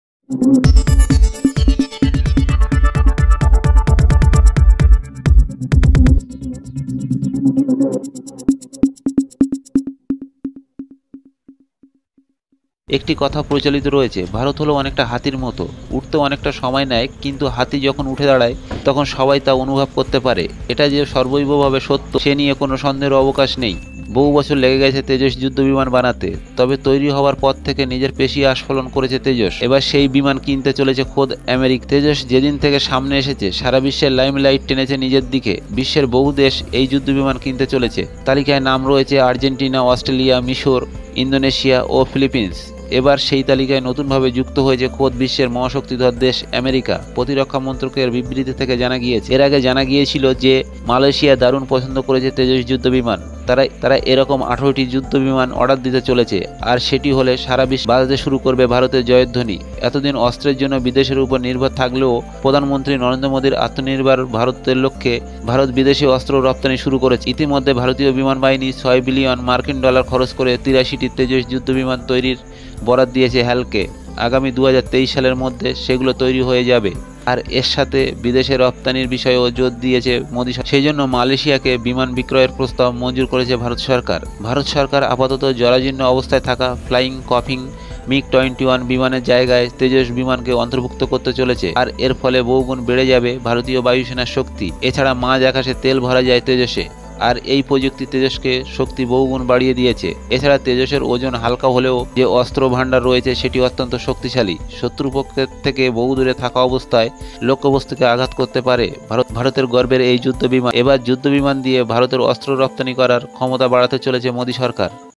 গোটা বিশ্বে ডঙ্কা বাজল ‘তেজস”-র।আমেরিকা, অস্ট্রেলিয়াও কিনতে চায় ভারতের যুদ্ধবিমান! News crowd.
channel site: NEWS CROWD In this channel we upload most surprising, shocking, amazing, mysterious facts about world and Space.